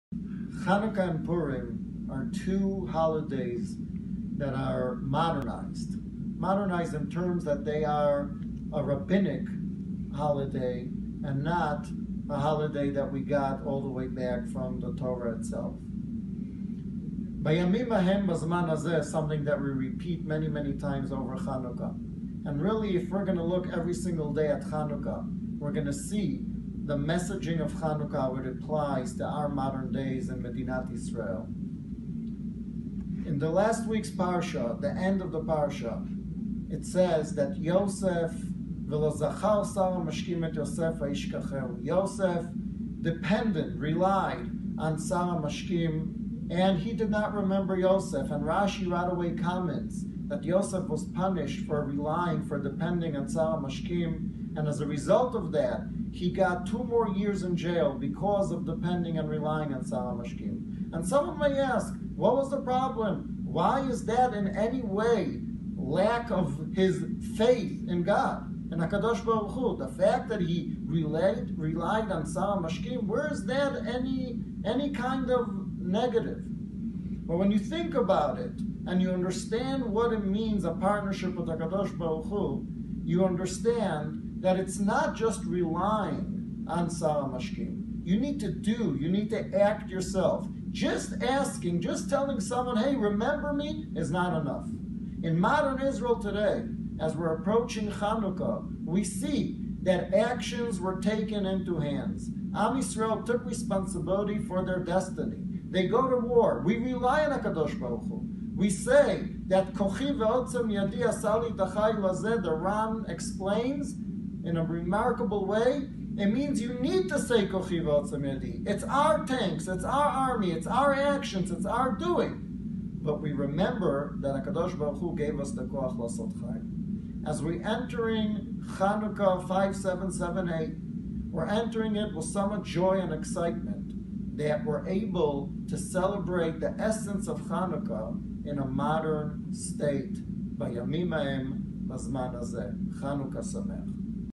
First Night of Chanukah
Dvar Torah by Bnei Akiva of the US and Canada's Director, Rav Shaul Feldman. Chanukah 5778